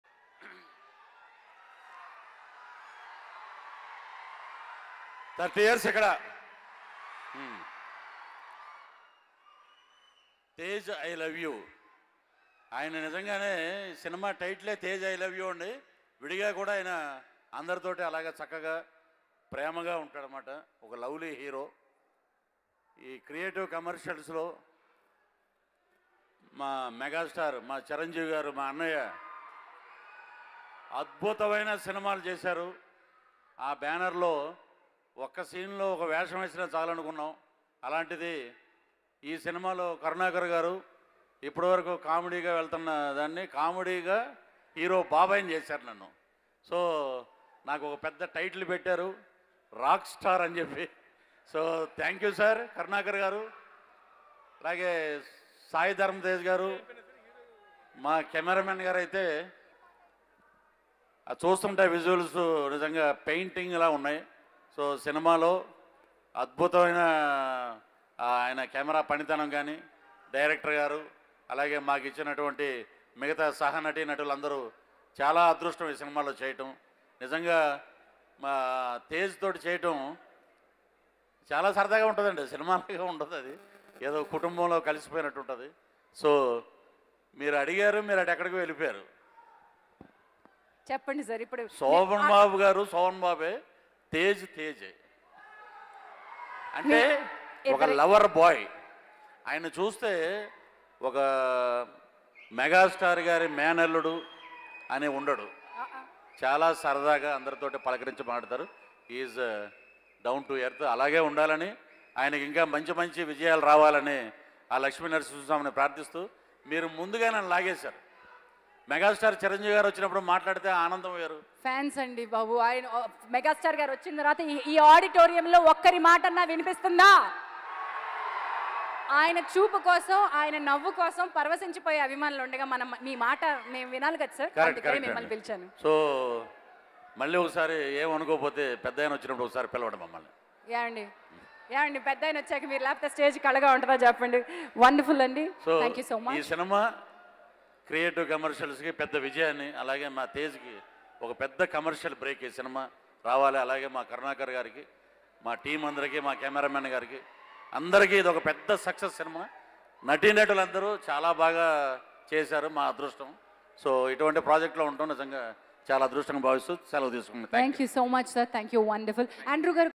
Comedian Prudhvi Raj Speech @ Tej I Love You Audio Launch
Watch TejILoveYou Audio Launch LIVE. Starring SaiDharamTej, AnupamaParameswaran. Music By GopiSundar, Directed By A.Karunakaran, Produced By KS Rama Rao Under The Banner of Creative Commercials Movie Makers.

Movie :  Tej I Love You
Banner : Creative Commercials Movie Makers.
Producer : KS Rama Rao.
Director : A.Karunakaran
Cast : Sai Dharam Tej, Anupama Parameswaran